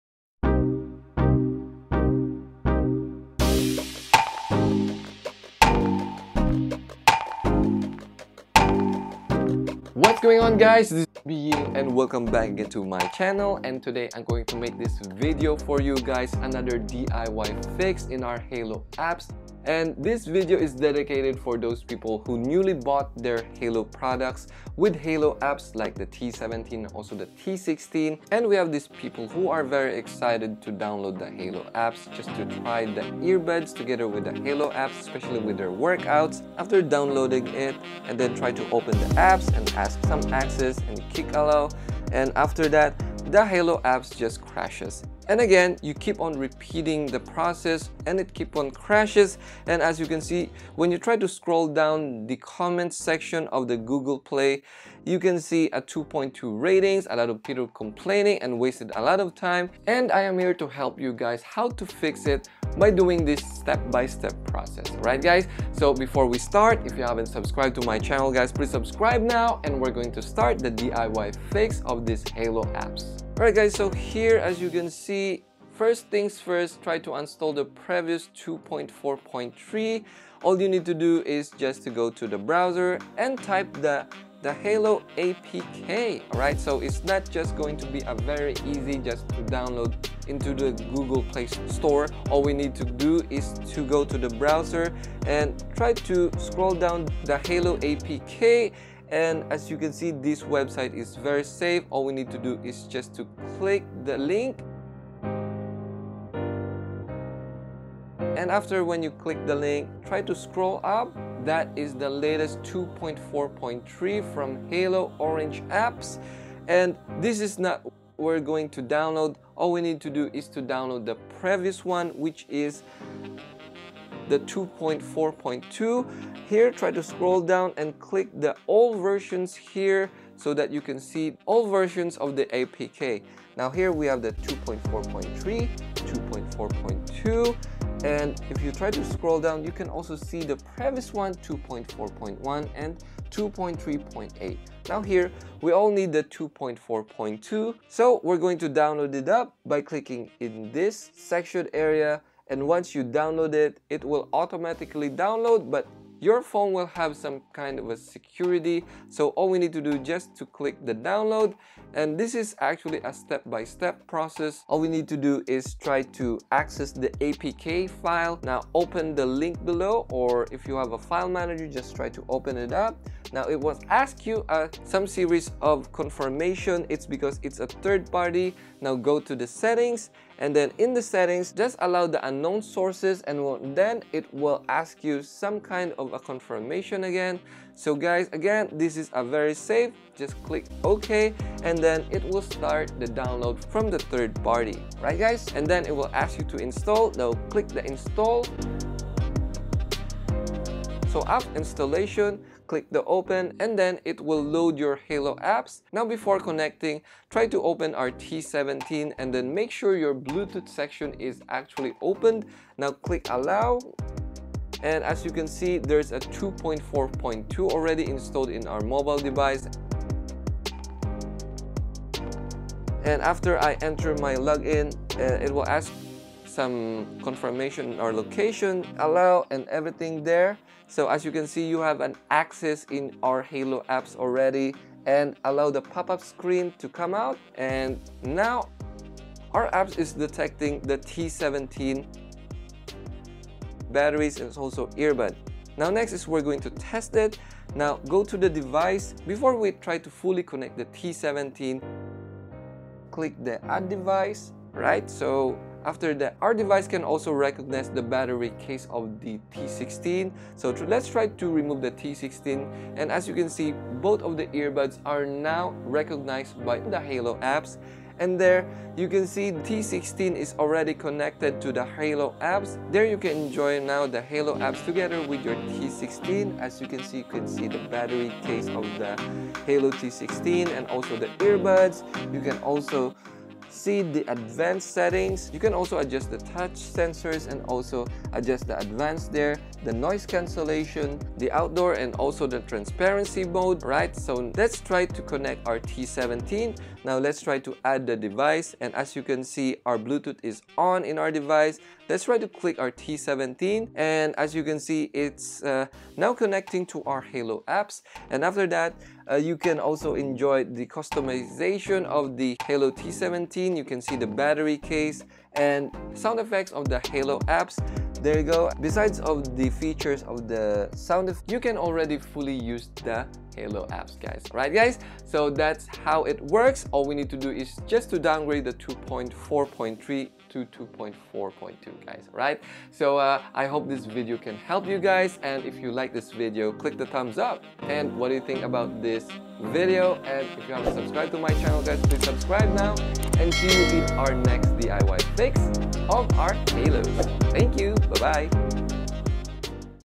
[HD] DIY FIX HAYLOU APPS - DOWNGRADE APPS - JUNE 4, 2021 UPDATED
This is a video of DIY fix of HAYLOU apps after the update on JUNE 4, 2021. Lower versions of Andriod 7 and Apples are not able to open the orange LOGO HAYLOU apps due to compatibility HAYLOU app versions 2.4.3. In this video I will show you how to use again your HAYLOU orange app. This is a step bu step process on restoring your previous settings apps. Hope this video can help you guys. Cheers!